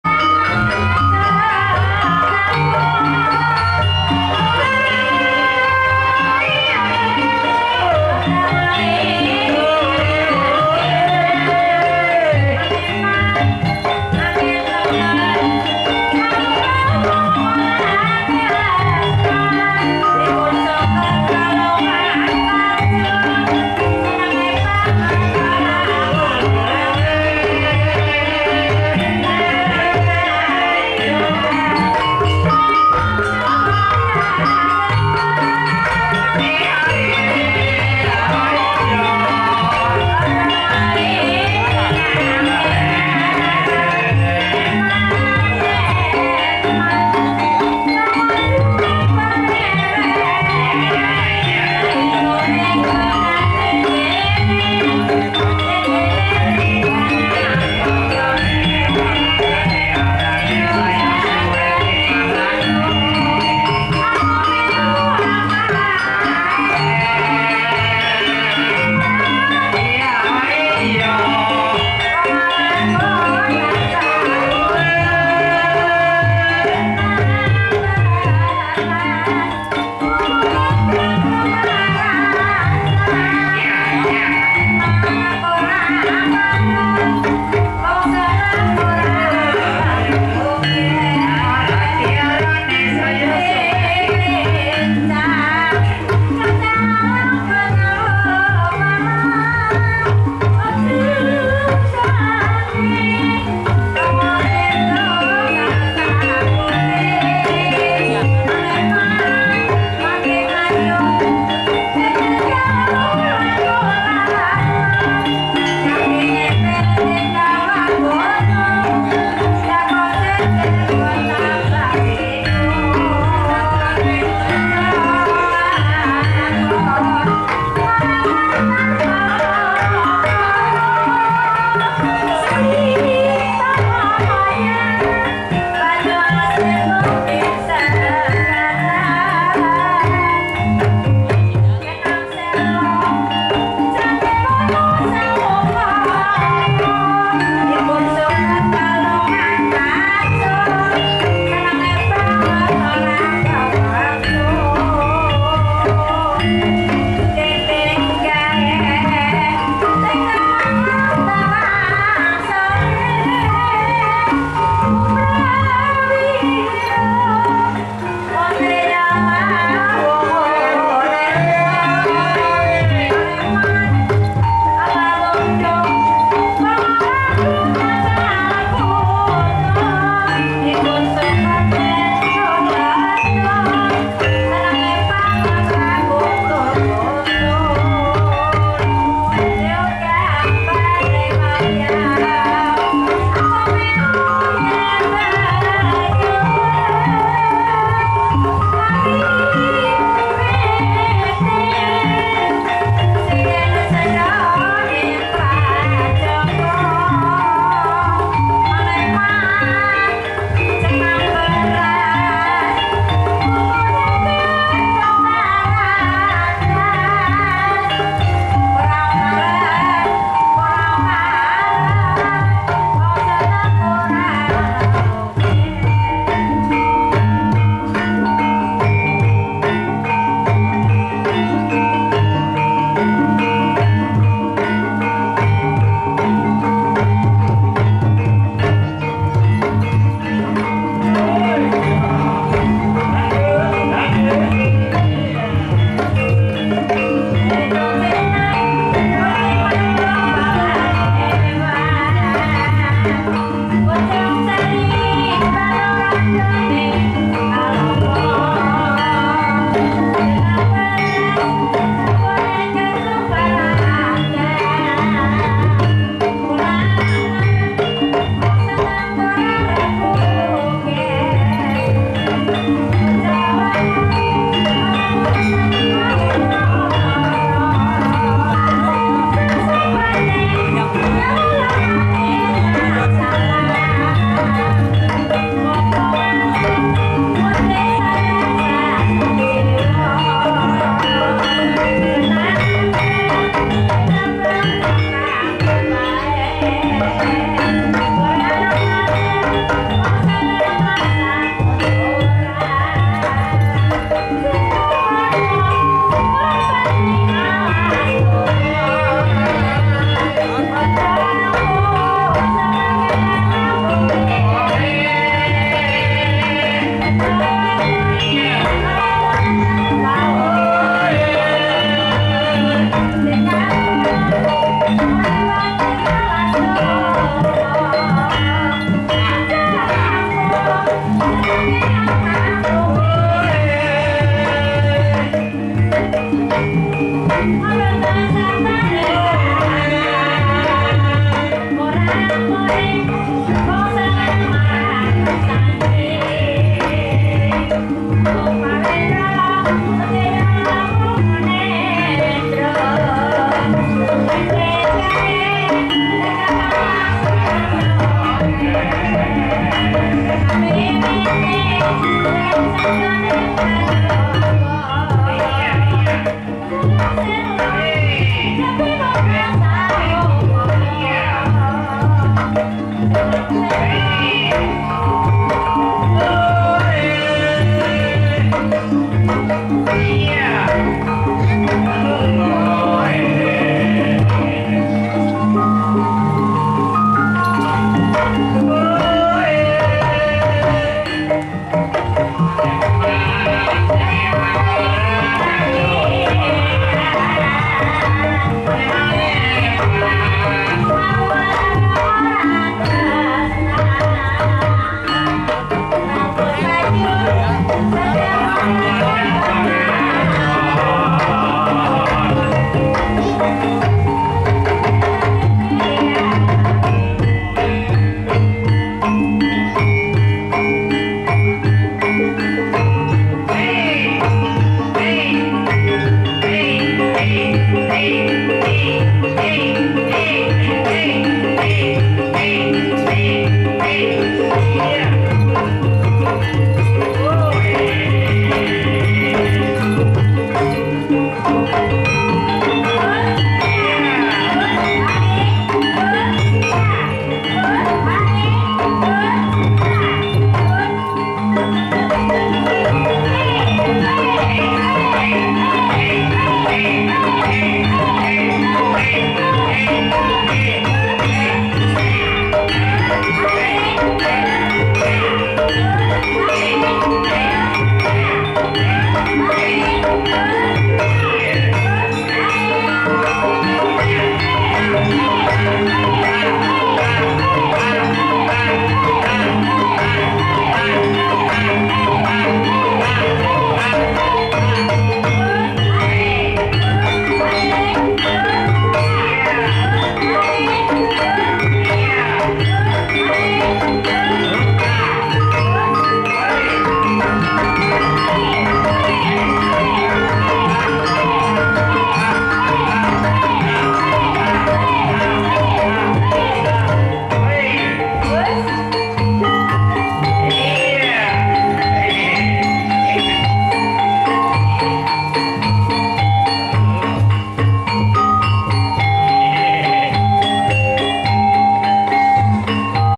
Cakilan Cantik Ebeg Banyumasan Terbaru Gumelem Banjarnegara
KUMPULAN EBEG TERBAIK  JAWA TENGAH

Ebeg adalah bentuk tari tradisional khas Banyumas dengan properti utama berupa ebeg atau kuda kepang. Kesenian ini menggambarkan kegagahan prajurit berkuda dengan segala atraksinya. Biasanya dalam pertunjukkan ebeg dilengkapi dengan atraksi barongan, penthul & cépét.

TONTON JUGA Pertarungan Janturan Buto Cakil Dan Padepokan Burung Garuda & Macan Tutul